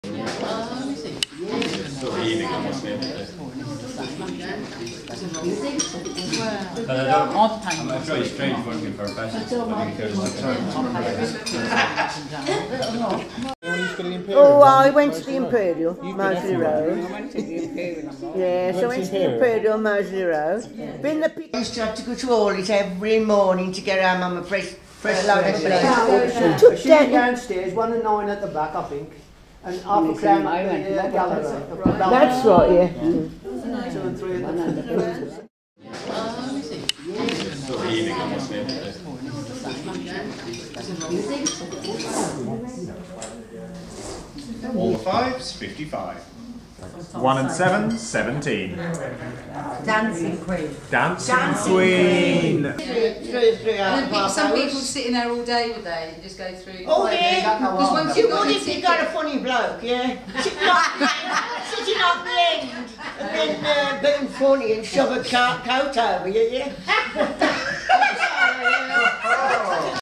Yard Talk 5
Highlights from Friction Arts' "Echoes" project. An event called 'Yard Talk' at The Edge in Digbeth Birmingham - 27th October 2012.